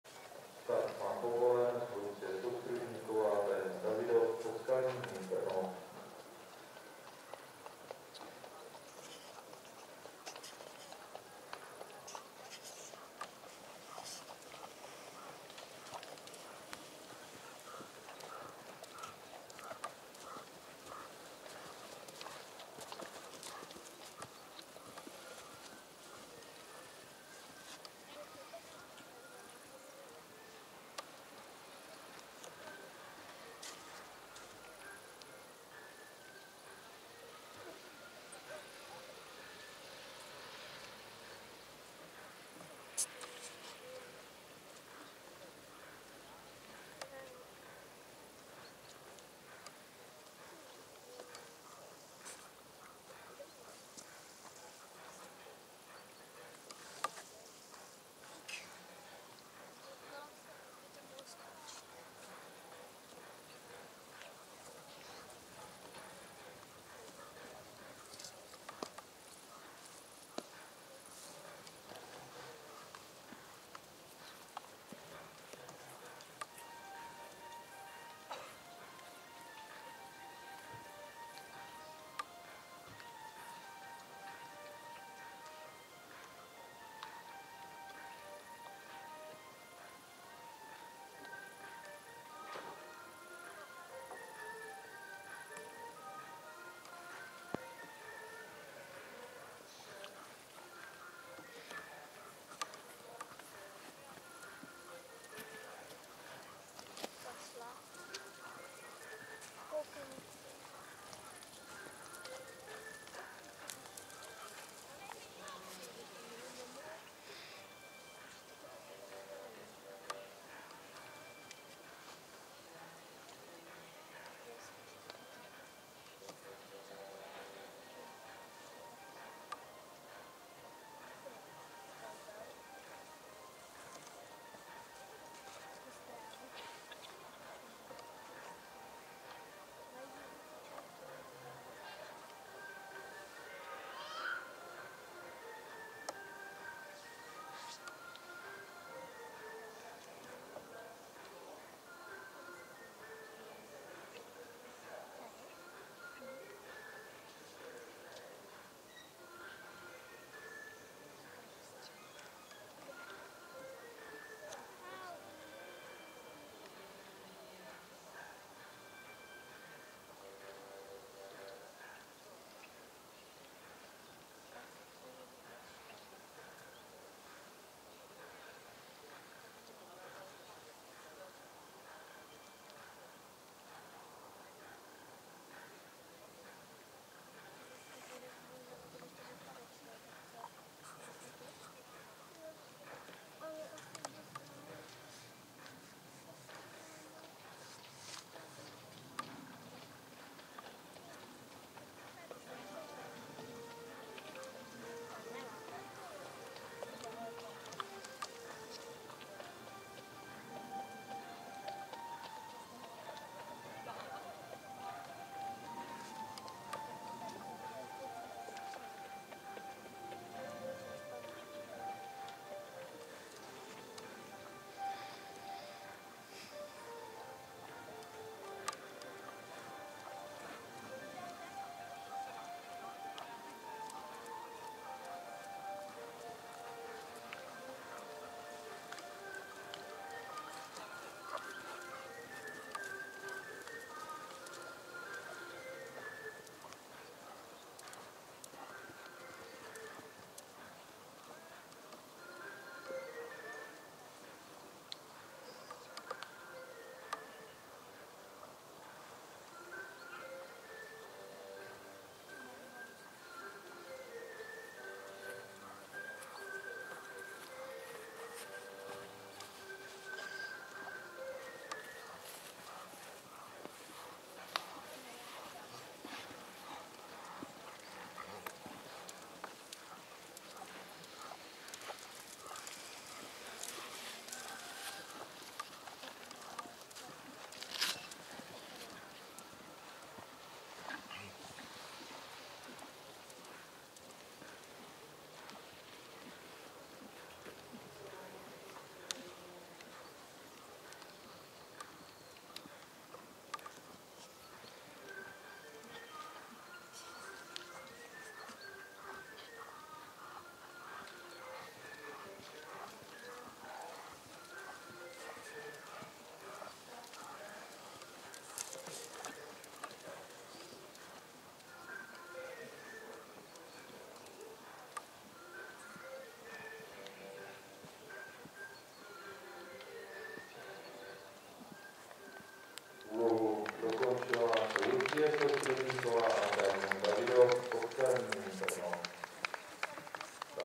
ČDP 2013 - Lucie Soustružníková / Diamond Davidowss / 1. kolo
Finále ČDP 2013 Panská Lícha / Czech Dressage Cup 2013 - final (Panska Licha)